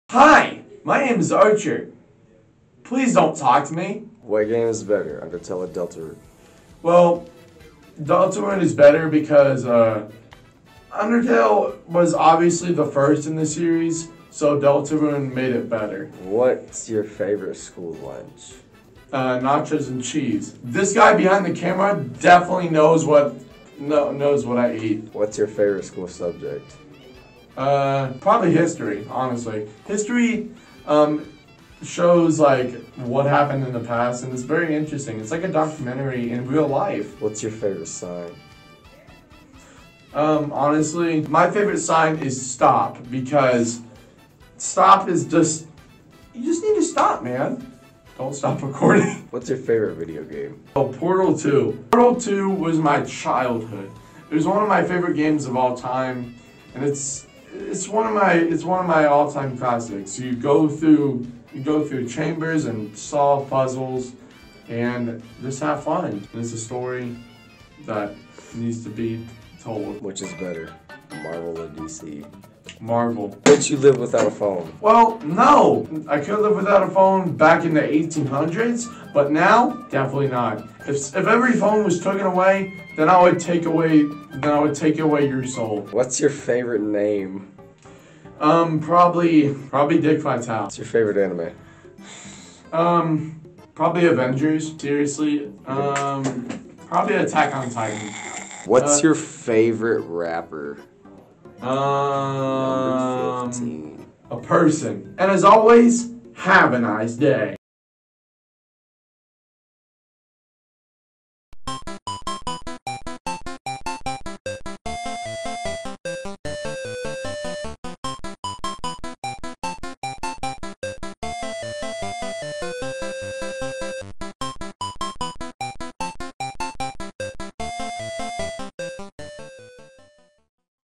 Archers interview(School Project)
Archer gets interviewed.

Chapters:
0:00 Intro
0:04 What Game is Better?
0:16 What's your favorite School Lunch?
0:25 What's your favorite School Subject?
0:37 What's your favorite Sign?
0:49 What's your favorite Video Game?
1:09 Which is better, Marvel or DC?
1:13 Could you live without a Phone?
1:25 What's your favorite Name?
1:30 What's your favorite Anime?
1:38 What's your favorite Rapper?
1:45 Conclusion
1:47 Credits
1:51 Outro